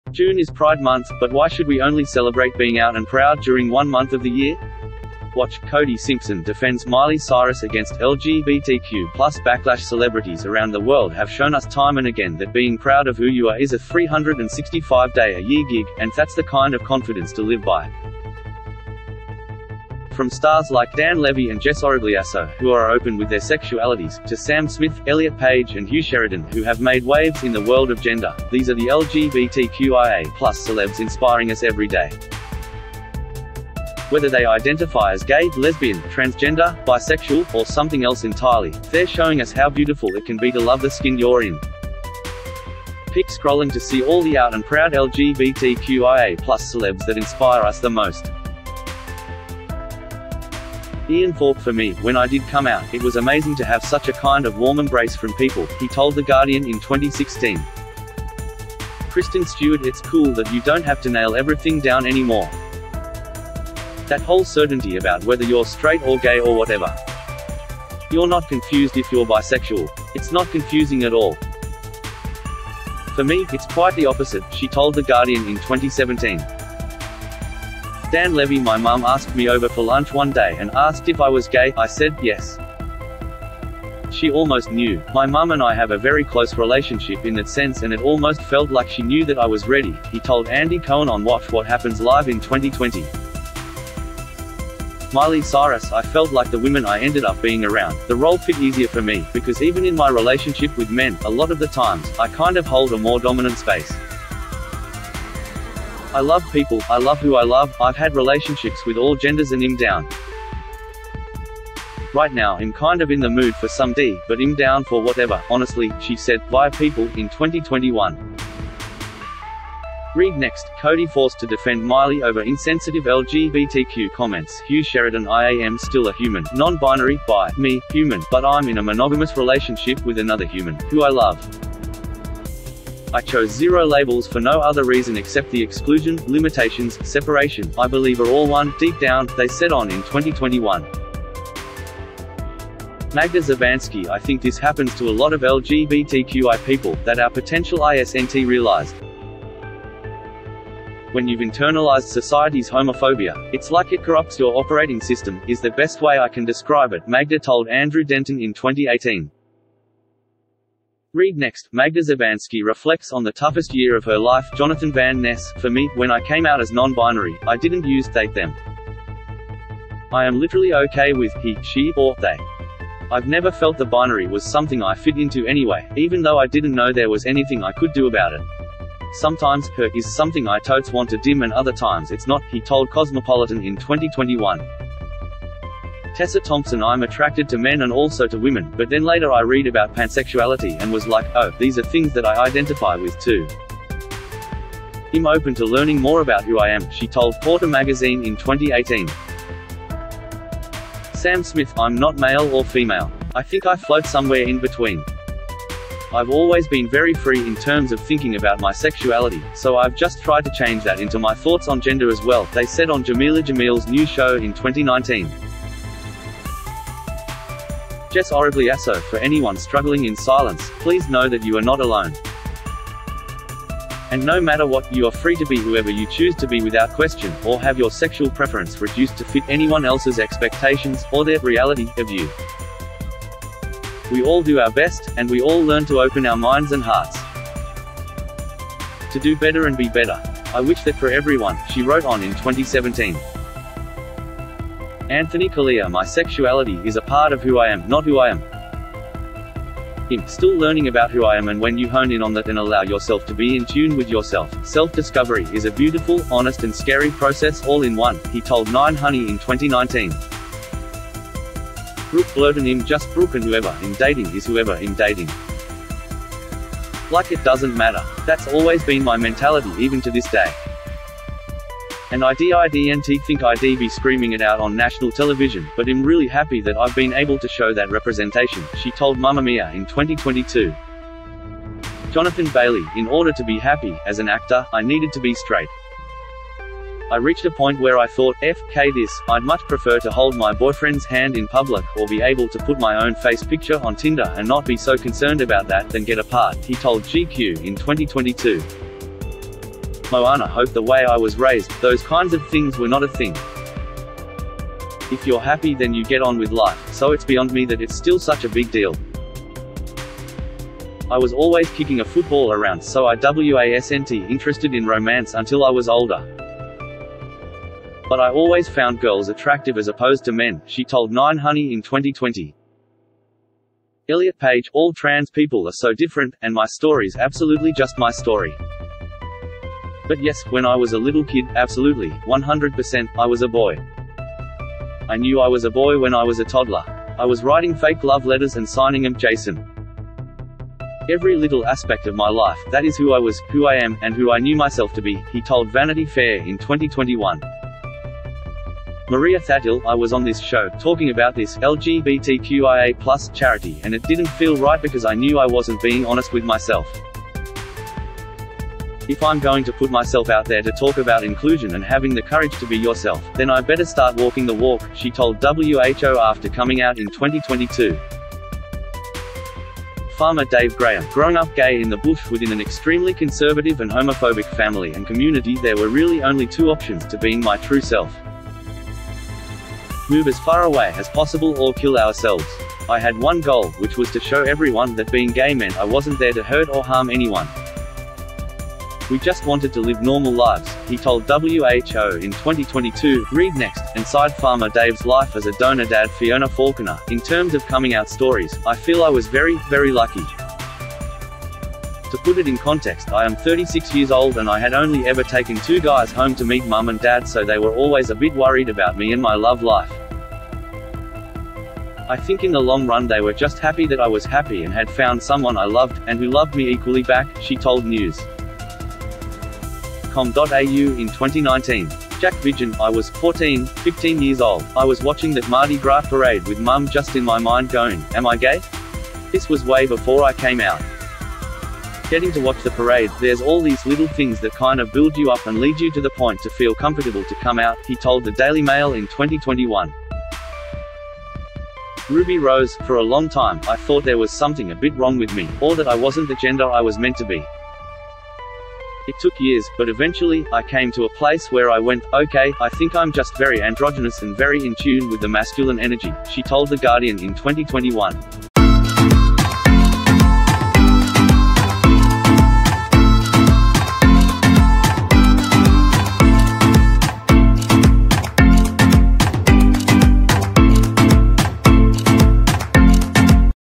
The out and proud LGBTQIA+ celebs that inspire us the most
June is Pride Month, but why should we only celebrate being out and proud during one month of the year? WATCH: Cody Simpson defends Miley Cyrus against LGBTQ+ backlash   Celebrities around the world have shown us time and again that being proud of who you are is a 365-day a year gig, and that's the kind of confidence to live by.   From stars like Dan Levy and Jess Origliasso who are open with their sexualities, to Sam Smith, Elliot Page and Hugh Sheridan who have made waves in the world of gender, these are the LGBTQIA+ celebs inspiring us every day.   Whether they identify as gay, lesbian, transgender, bisexual, or something else entirely, they're showing us how beautiful it can be to love the skin you're in.   Keep scrolling to see all the out and proud LGBTQIA+ celebs that inspire us the most.   Ian Thorpe  "For me, when I did come out, it was amazing to have such a kind of warm embrace from people," he told The Guardian in 2016.   Kristen Stewart  "It’s cool that you don’t have to nail everything down any more. That whole certainty about whether you’re straight or gay or whatever ... You’re not confused if you’re bisexual. It’s not confusing at all. For me, it’s quite the opposite," she told The Guardian in 2017.   Dan Levy  "My mum asked me over for lunch one day and [asked if I was gay] I said, 'Yes,' ... She almost knew. My mum and I have a very close relationship in that sense and it almost felt like she knew that I was ready," he told Andy Cohen on Watch What Happens Live in 2020.   Miley Cyrus  "I felt like the women I ended up being around, the role fit easier for me, because even in my relationship with men, a lot of the times, I kind of hold a more dominant space ... I love people, I love who I love, I've had relationships with all genders and I'm down. Right now I'm kind of in the mood for some D, but I'm down for whatever, honestly," she said, via People, in 2021.  READ NEXT: Cody forced to defend Miley over "insensitive" LGBTQ comments   Hugh Sheridan "I AM still a human (non binary/bi/me/Hughman) but I’m in a monogamous relationship with another human, who I love ... I chose zero labels for no other reason except the exclusion, limitations, separation, I believe are all one, deeeeep down," they said on  in 2021.   Magda Szubanski  "I think this happens to a lot of LGBTQI people, that our potential isn't realised. When you've internalised society's homophobia, it's like it corrupts your operating system, is the best way I can describe it," Magda told Andrew Denton in 2018.  READ NEXT: Magda Szubanski reflects on the toughest year of her life   Jonathan Van Ness  “For me, when I came out as non-binary, I didn’t use ‘they/them’. I am literally OK with ‘he’, ‘she’ or ‘they’. I’ve never felt the binary was something I fit into anyway, even though I didn’t know there was anything I could do about it… Sometimes ‘her’ is something I totes want to dim and other times it’s not," he told Cosmopolitan in 2021.   Tessa Thompson   "I’m attrac